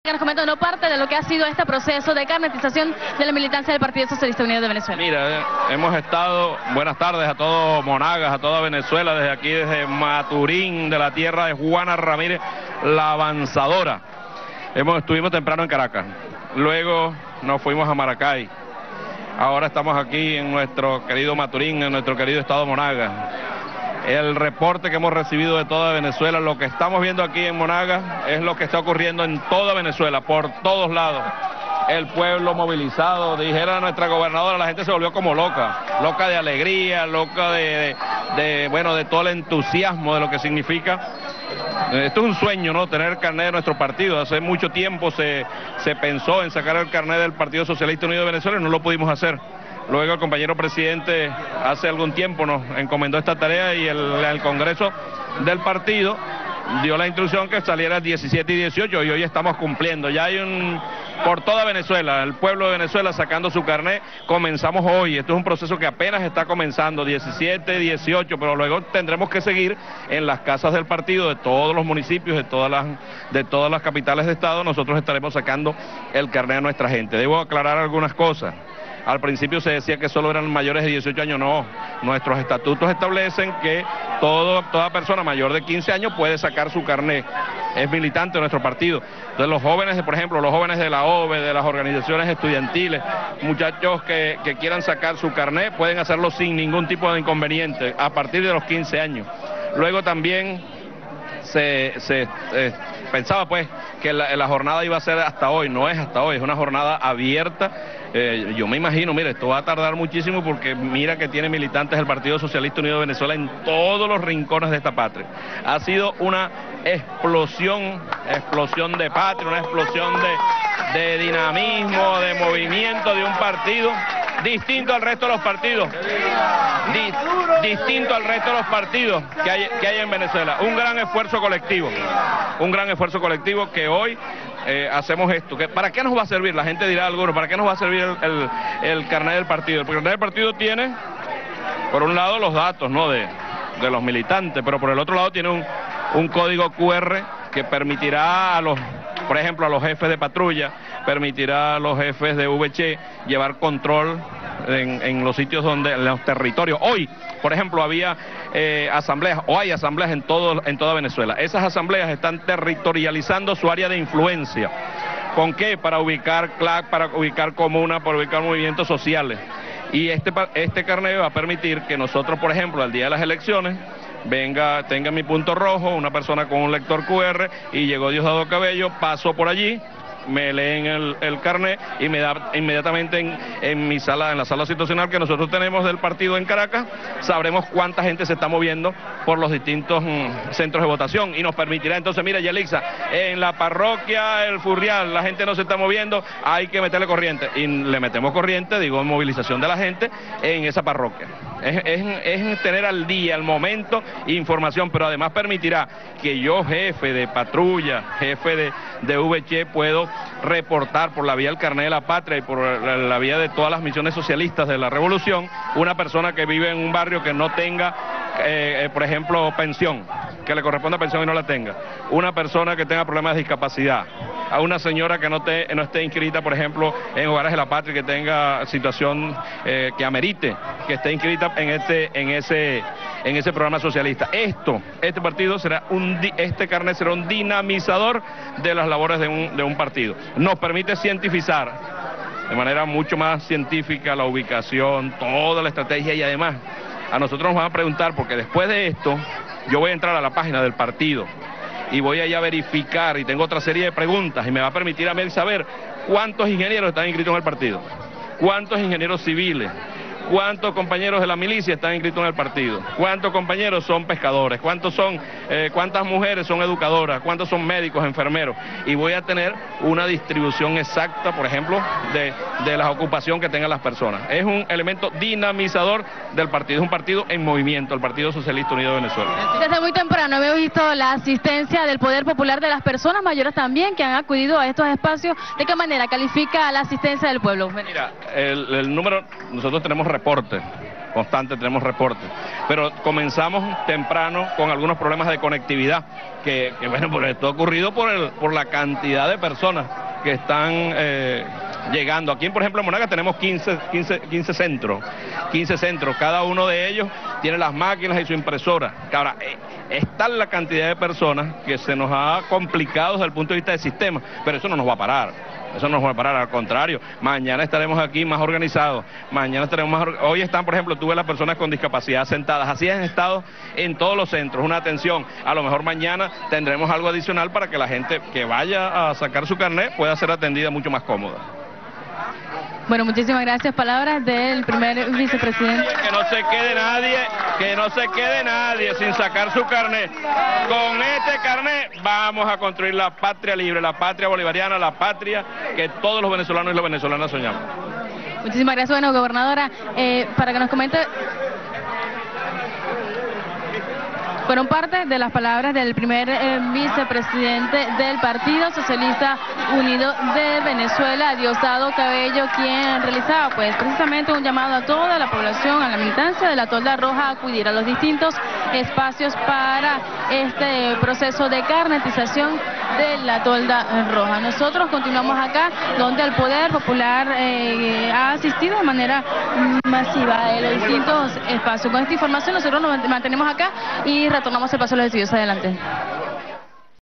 Diosdado Cabello desde Maturín: Carnetización del PSUV seguirá abierta por un buen tiempo
El vicepresidente del PSUV, Diosdado Cabello, se expresa en torno a la carnetización del partido e indica que esta durará un largo tiempo debido a la gran cantidad de militantes del partido. También explica que se busca sistematizar la movilización de militantes y conocer la cantidad de ingenieros, técnicos y personas de diferentes profesiones que son miembros del partido.